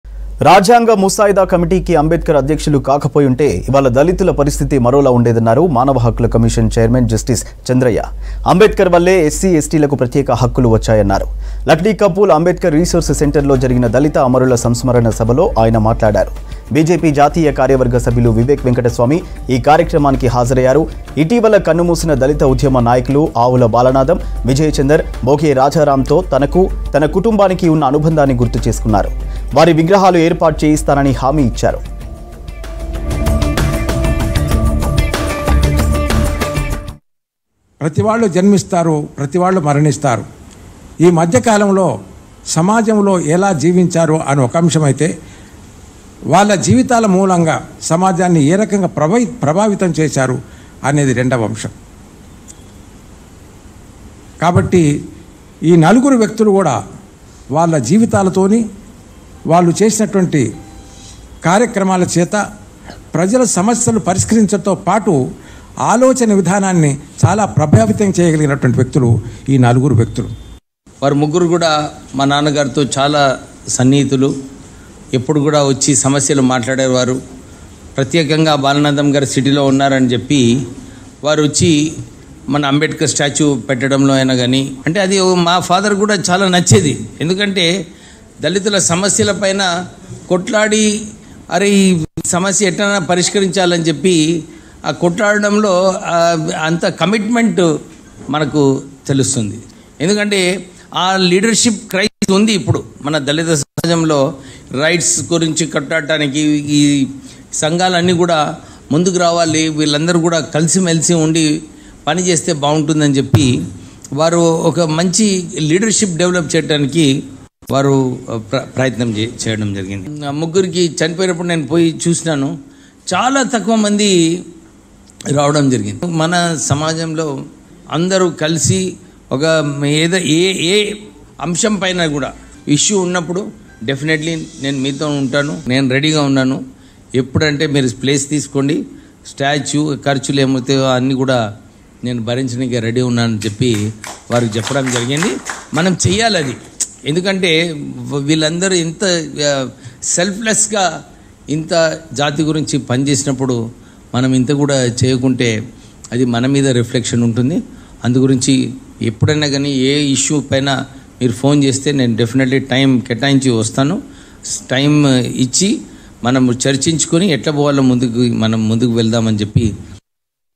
BJP Leader Vivek Venkataswamy, JusticeChandraiah Dalit Participates In Dalit Martyrs MemorialMeeting
BJP National Executive Committee Member Vivek Venkataswamy, Justice Chandraiah Dalit Participates In Dalit Martyrs Memorial Meeting. Vivek Venkataswamy Vows to Install Statues Dalit Leaders Statues

లైసెన్స్ తెచ్చిన లక్కు..